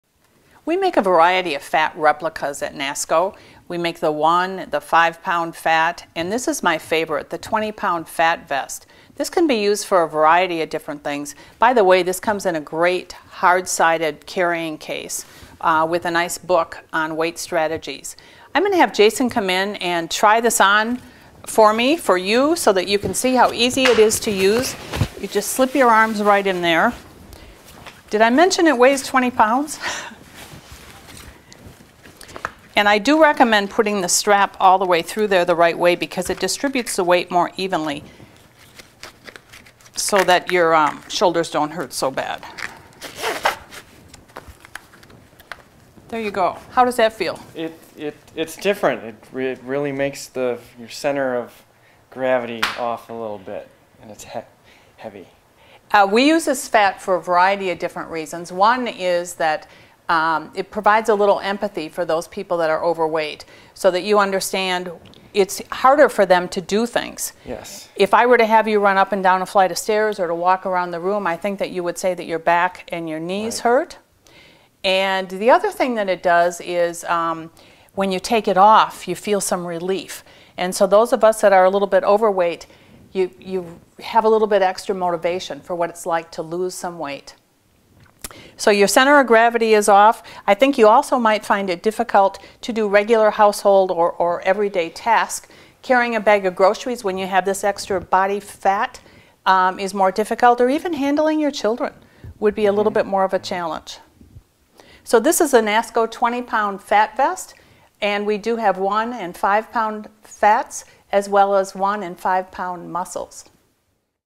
20-lb. Body Fat Vest from Nasco
Body Fat Vest with Booklet and Case

A specially designed vest that allows you to temporarily experience 20 lbs. of additional body fat. Just slip into this garment for 10 minutes to experience some of the physical changes that occur with weight gain, such as stress and strain on the back and knees. Aids in understanding and empathy with the issues of obesity. Includes the 20-lb. Body Fat Vest with heavy webbed straps, a publication entitled Weight Management Education Strategies (WA21219HR), and carrying case.

Connect with Nasco Education!